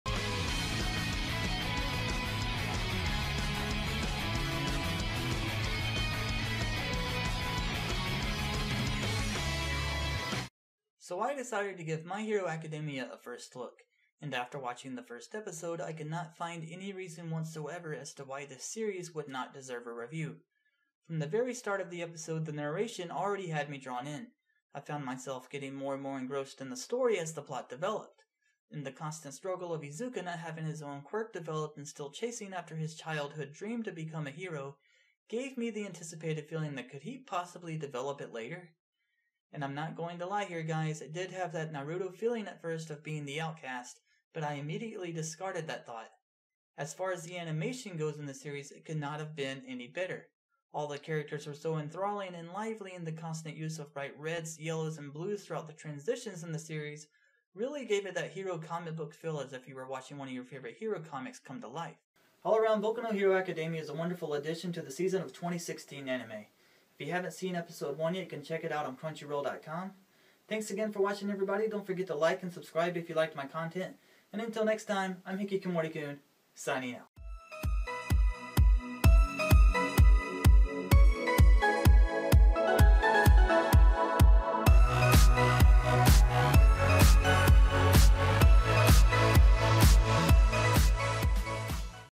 Otaku Diet OD:BOKU NO HERO ACADEMIA: 1ST IMPRESSION
An Otaku Diet first look of episode one of Boku no hero Academia
Sorry for the horrible sound quality. Working on getting better equipment.